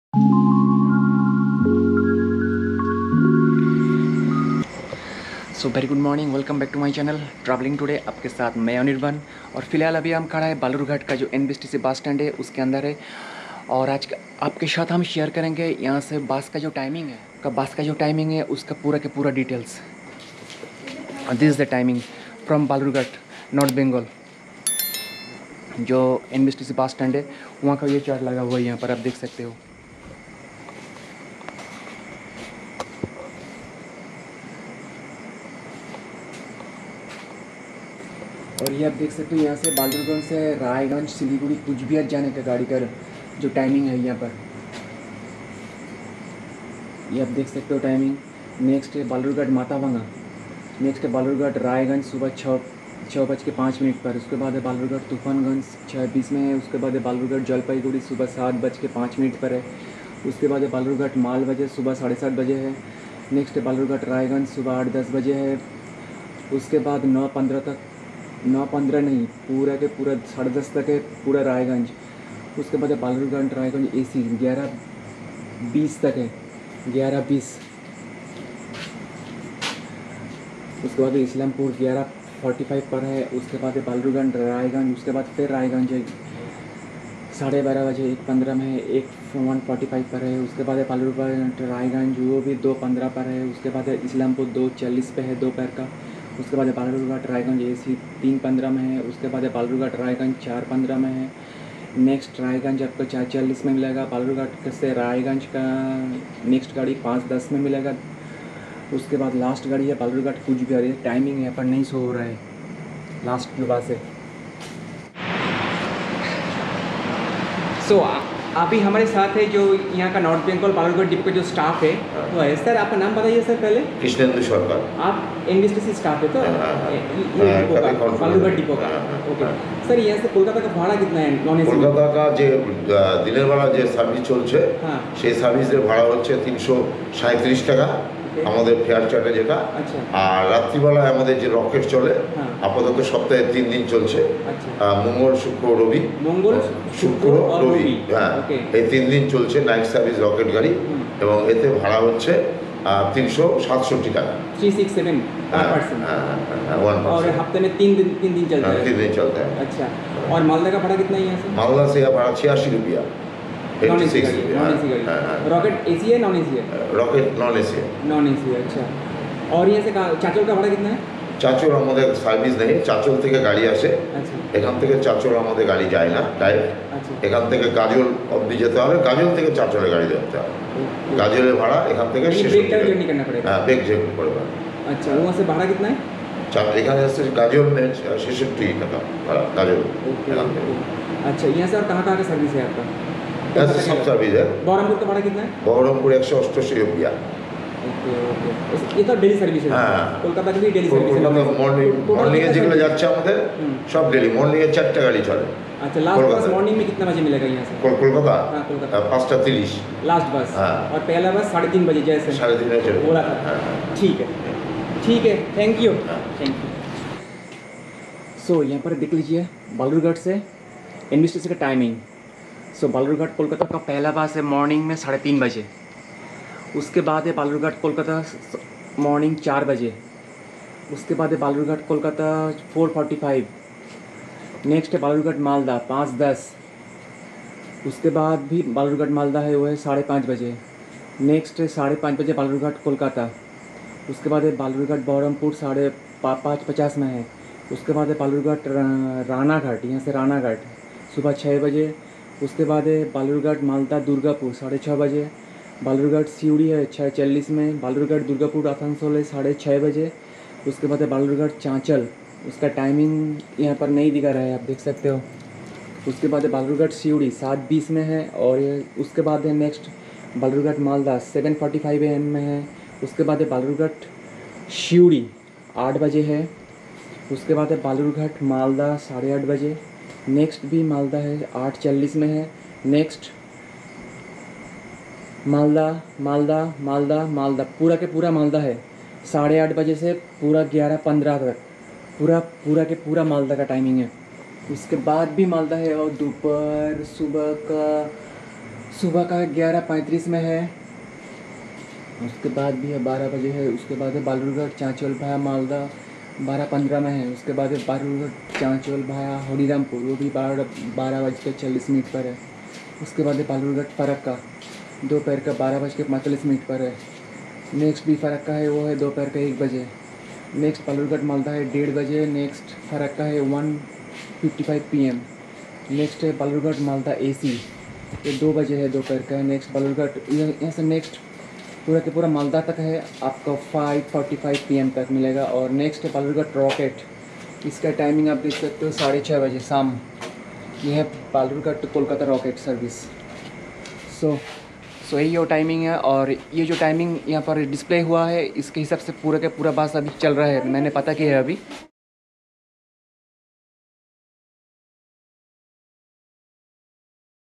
NBSTC Bus Stand Balurghat | State Bus Timings From Balurghat| Traveling 2 day
Hii...Everyone

You will get lots of Bus for Kolkata, Malda,Siliguri, Baharampur from Balurghat.